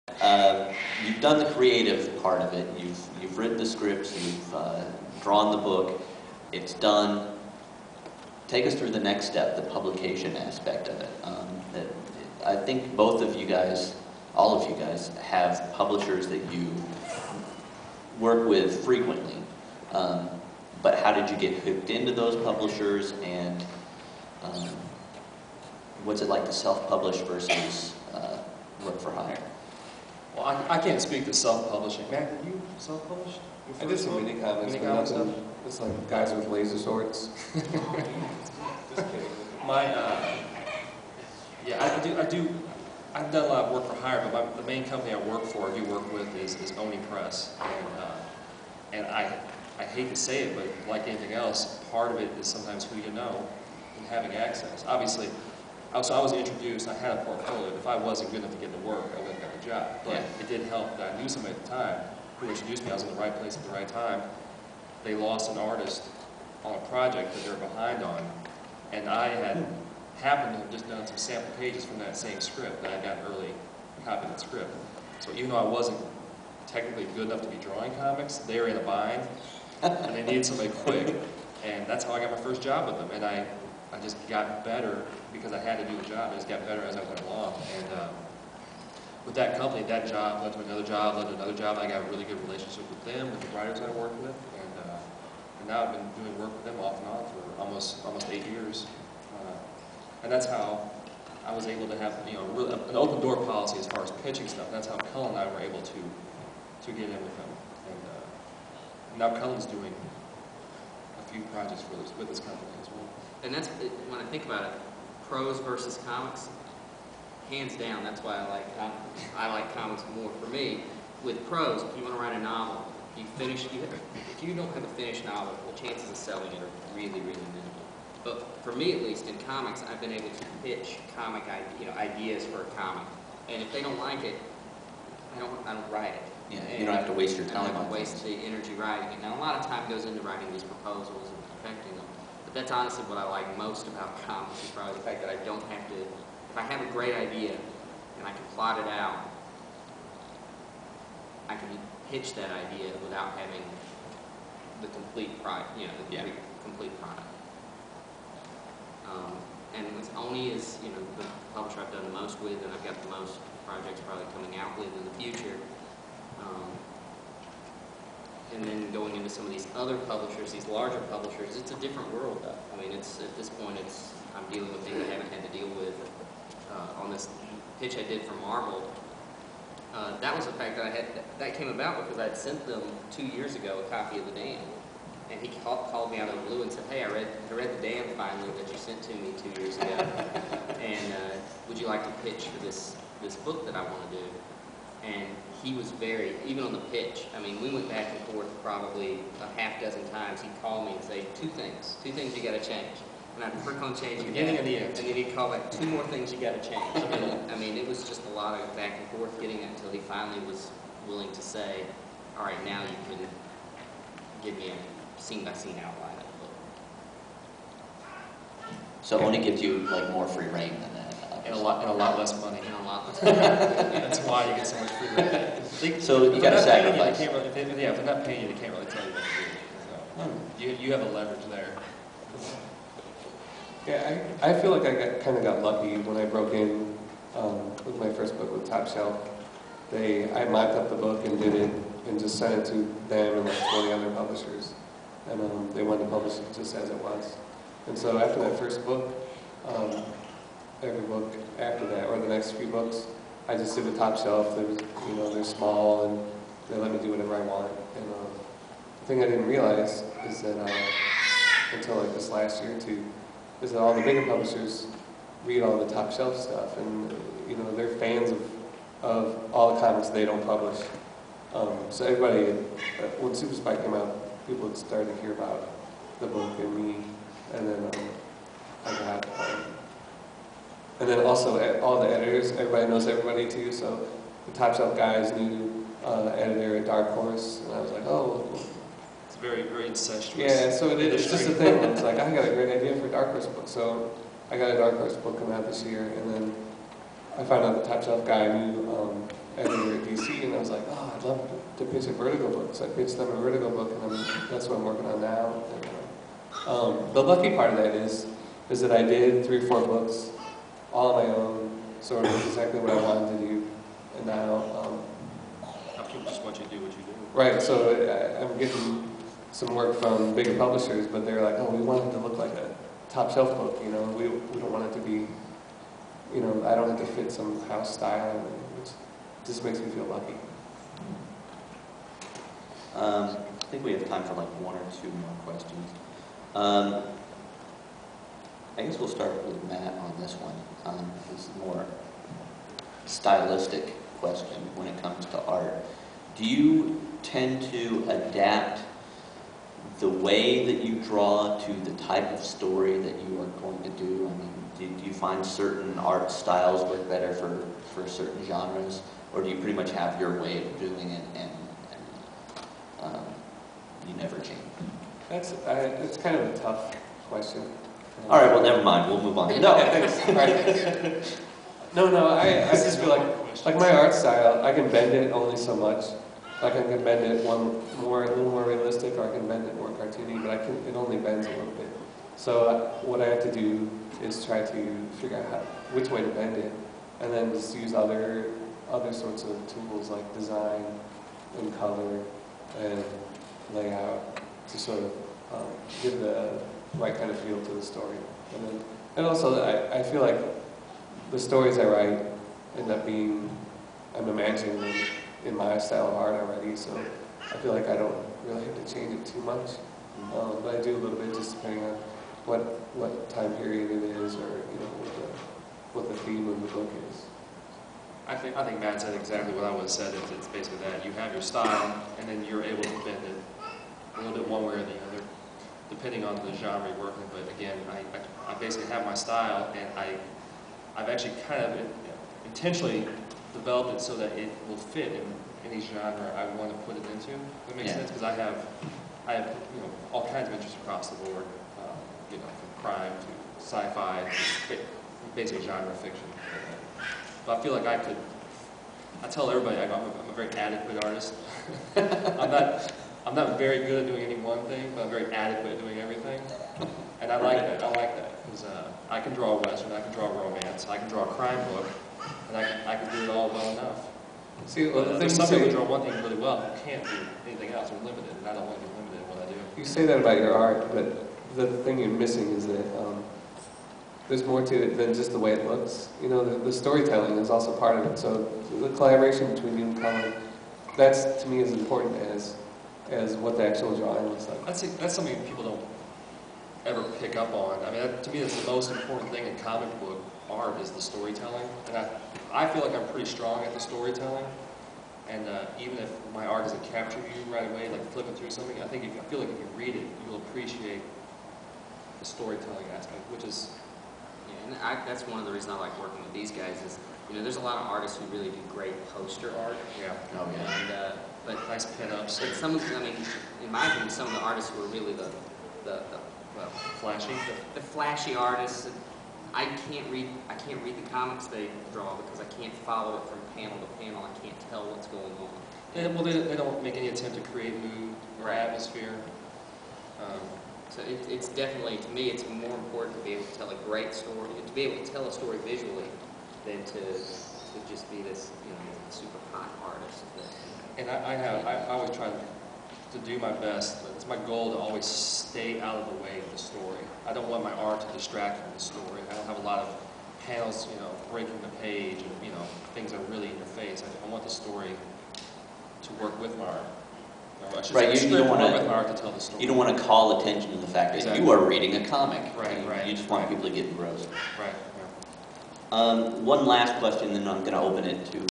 comics lecture 2009 6
On Friday, April 24, 2009, I hosted a panel discussion on comics in the Olin Theater on the campus of Lewis and Clark Community College in Godfrey, IL.

My three guest speakers were Cullen Bunn, Brian Hurtt, and Matt Kindt.

This is part six of the hour long lecture.